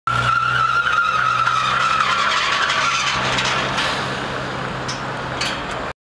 Fairway London taxi burnout wheelspin
Not a real burnout! This taxi was just pulling away at the same time a steel barrier was being moved.